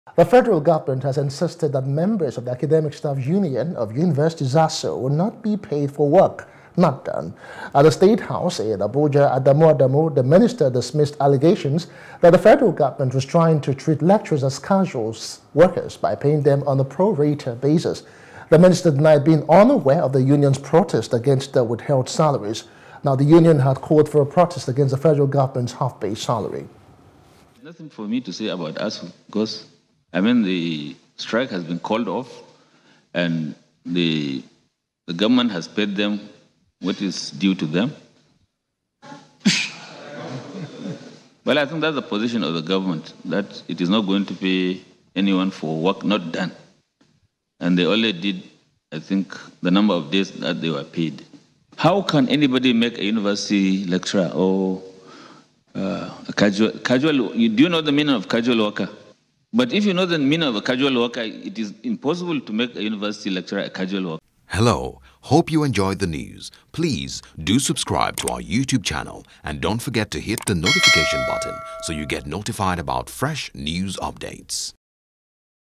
Half Salary: We Won’t Pay Lecturers For Work Not Done, F.G. Insists | NEWS
The federal government has insisted that members of the Academic Staff Union of Universities (ASUU) will not be paid for work not done.